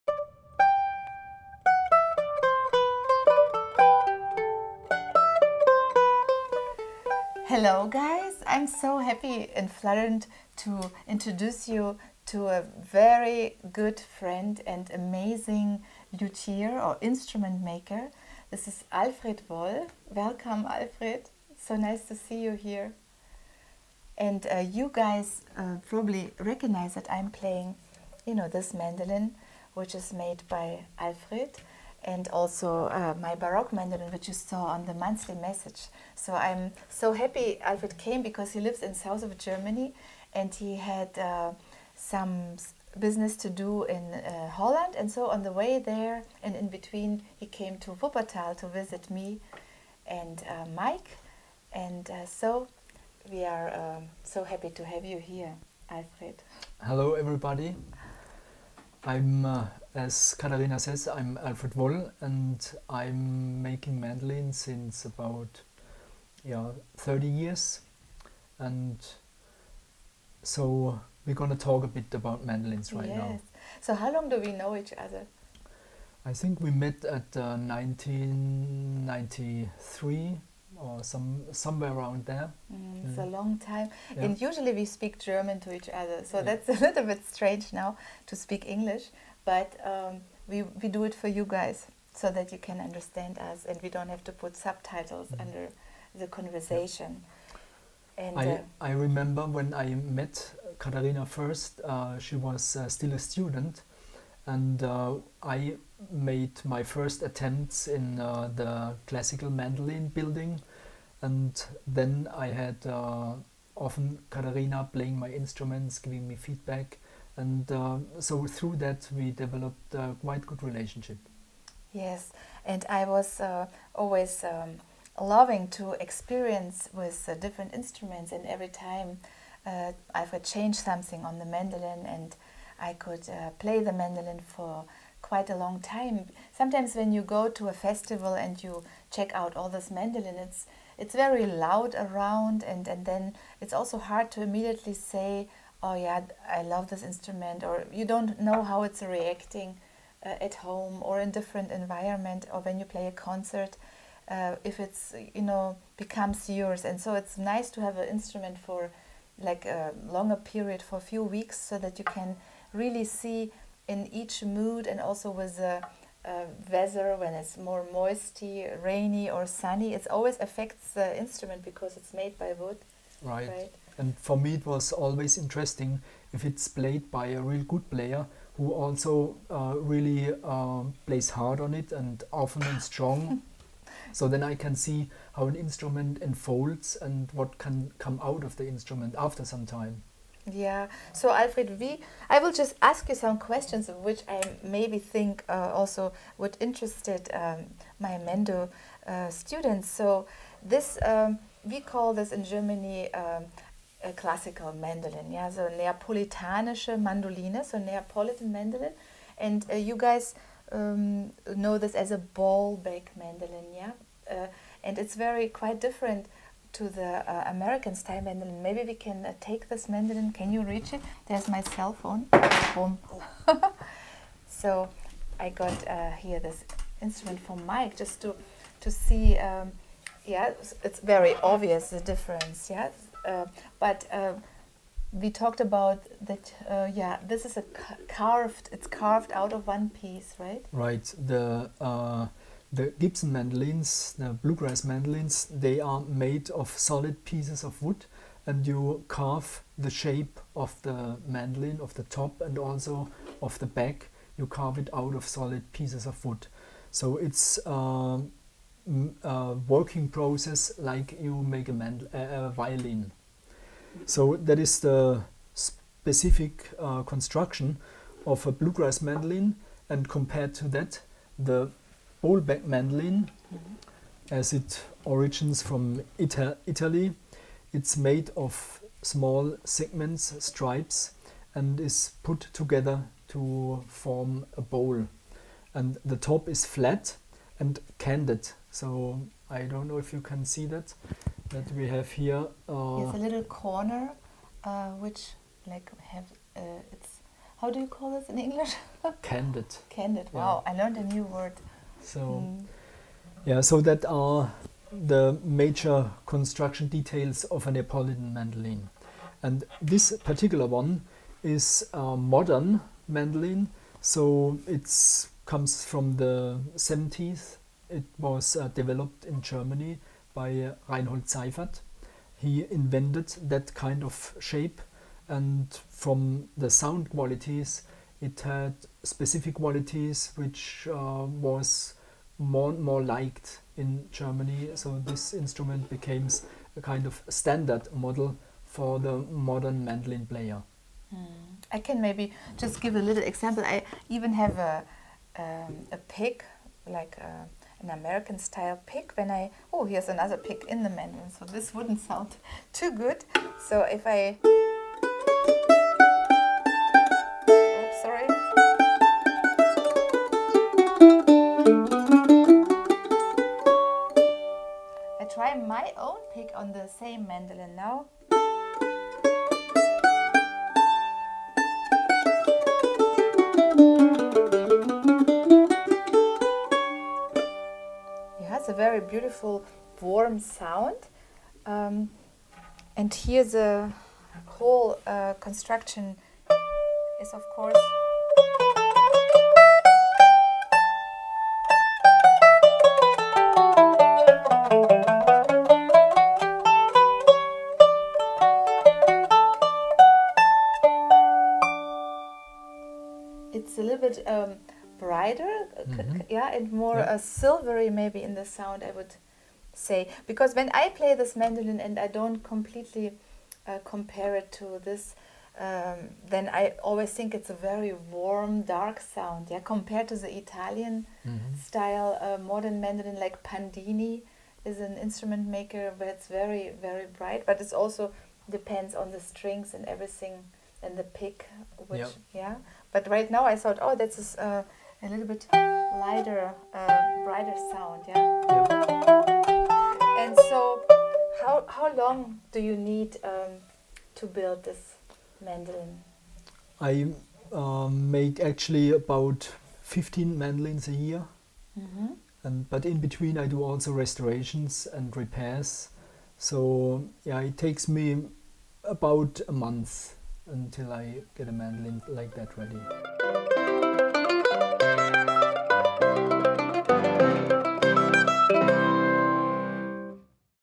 Caterina Lichtenberg talks with Mandolin Luthier Alfred Woll
ArtistWorks classical mandolin teacher Caterina Lichtenberg talks with renowned Mandolin luthier Alfred Woll - the maker of her primary bowl back and baroque mandolins used in her online lessons at ArtistWorks. In a 6 part interview recorded at her home studio in Germany, Alfred and Caterina showcase, discuss and compare many mandolin styles and models dating back to 1752. This is just the first part, sign up for her online school at ArtistWorks to see the rest. Click the link for more info and free sample lessons